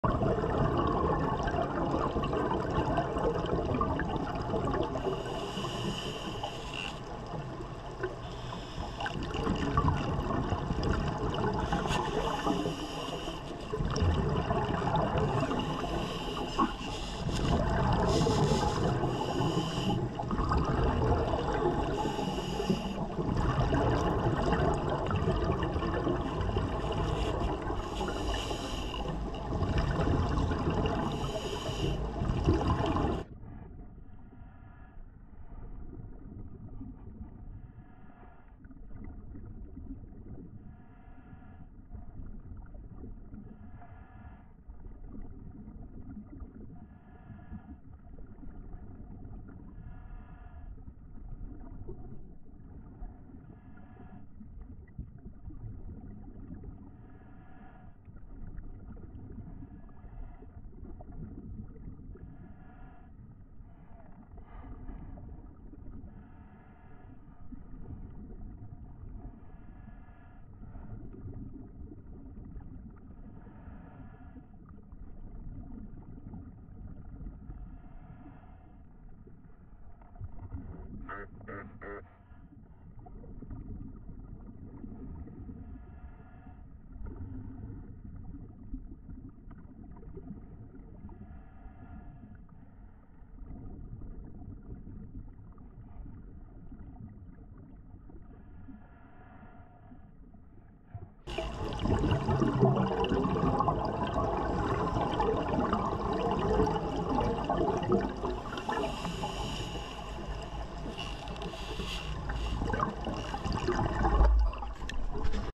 Emma the Tiger Shark
Emma is the most filmed, photographed, and famous shark on the planet. She has starred in or appeared in approximately 14 movies filmed by her closest friend Jim Abernethy.    Diving with her was probably the coolest thing I’ve ever done. She is so friendly and a fantastic shark to photograph. I’ve never had a Tiger Shark swim right up to me as she does. She is definitely a supermodel shark. Video by Steve Kathrein. Video by Steve Kathrein